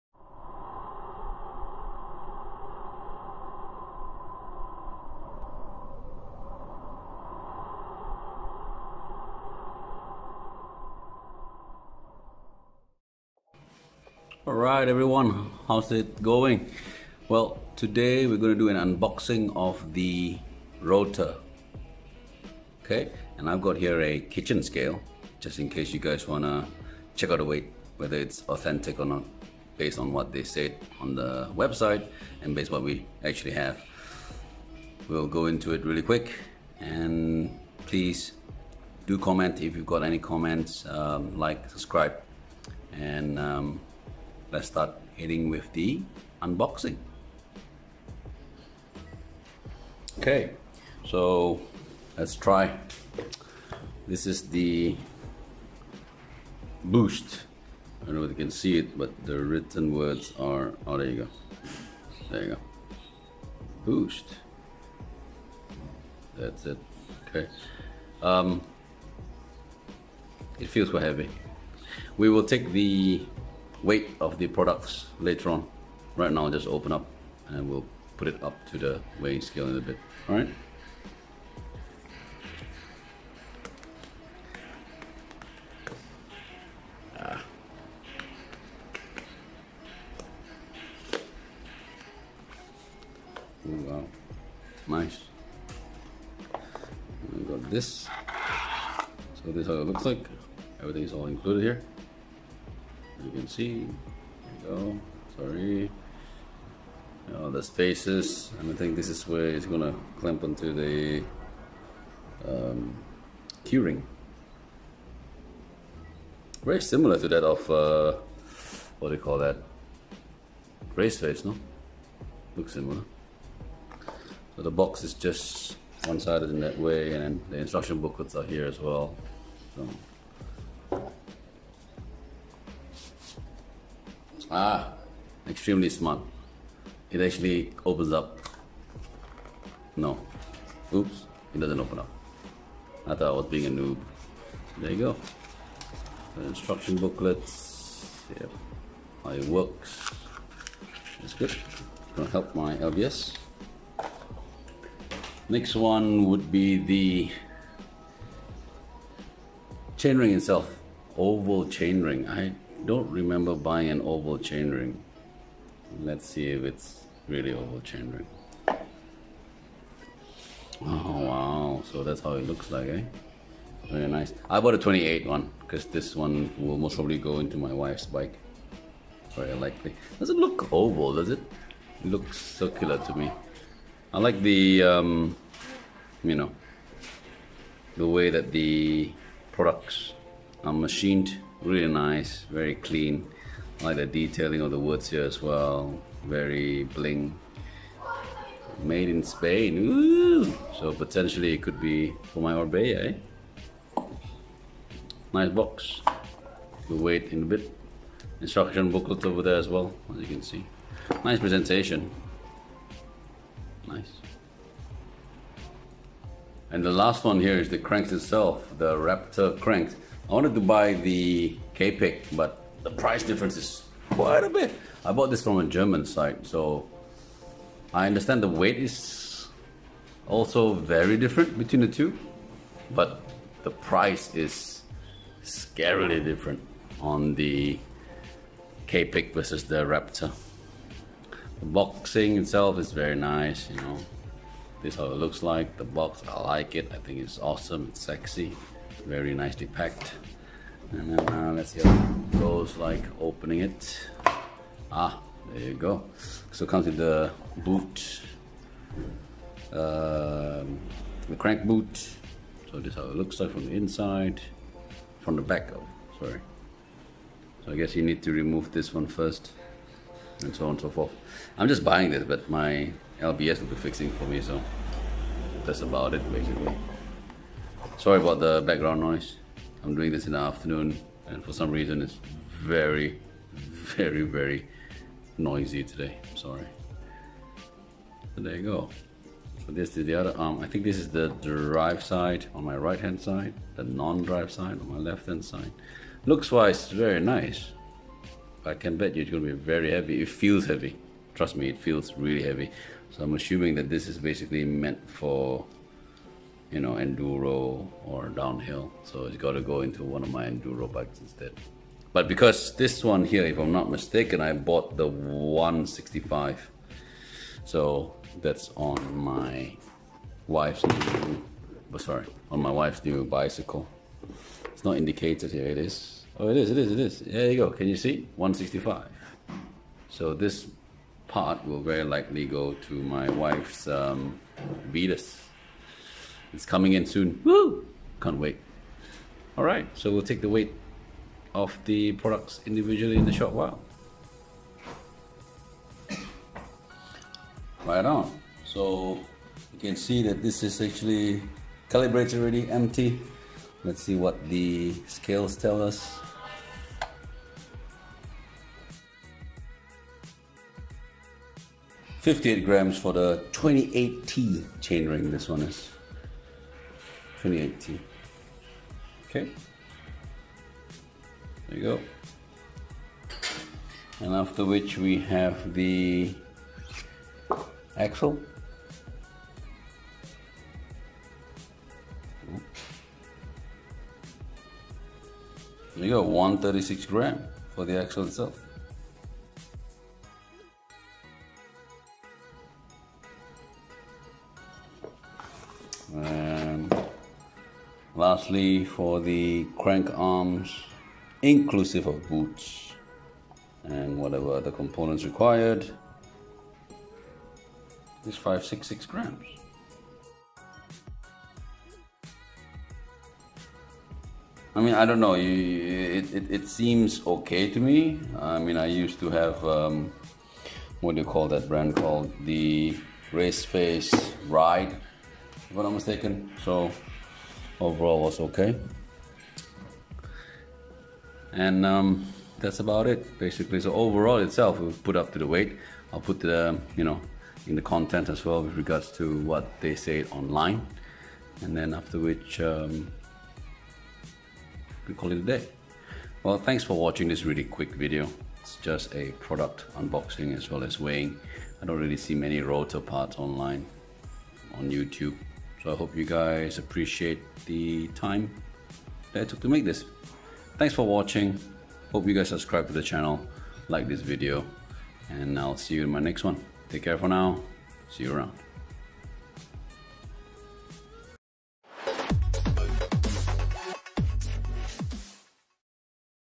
ROTOR R-RAPTOR CRANKSET (WEIGHT - UNBOXING!!! 🤩🤩🤩
Hello peeps!
Keeping it safe? Keeping it clean? Keeping your distance? I hope you all are.

It's time for an UNBOXING video this time BUT! of product and not a bike - my Christmas gift to myself has yet to arrive 😭

So in December, I found out that my Raceface Next SL G4 had a crack at the pedal insert and so, whilst waiting for my replacement to come, I decided to try out this Rotor crank - I have always wanted to try this brand!

There will be an update in the future of this product and I hope that the unboxing has helped you as well.

In the meantime, stay safe and ride safe there. Always mask up and keep your social distancing game strong!

And REMEMBER!!! There is no such thing as a shit bike as long as it makes you smile!!! 😄😄😄

Hardware: Huawei P30 Lite
Software: YouCut App / Windows Video Maker